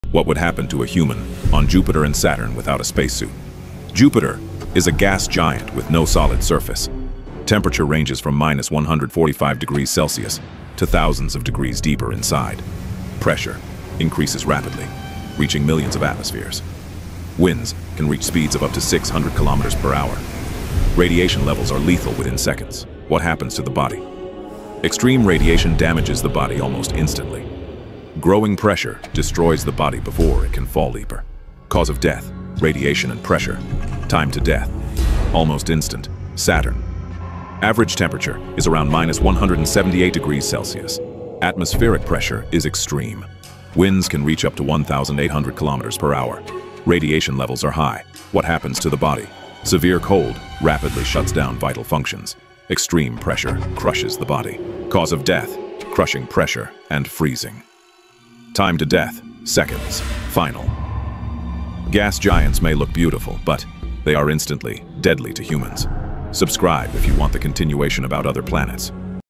Survival Time on different Planets - Part 2
Description for YouTube Video/Shorts (in English):
"What happens if you fall into Jupiter or Saturn without a spacesuit? 😱 These gas giants have NO solid surface – and survival is impossible!
Jupiter: Plunging through 1000 km/h winds, crushed by pressure 1000x Earth's, temperatures rising to thousands of degrees... you'd be vaporized long before reaching the "core" in seconds to minutes.
Saturn: Similar fate – deadly ammonia storms, -140°C at the clouds, then crushing pressure and heat turning you to plasma in minutes.
No air, no ground, just endless crushing gas hell.
Gas giants don't forgive – you'd be gone faster than you can blink!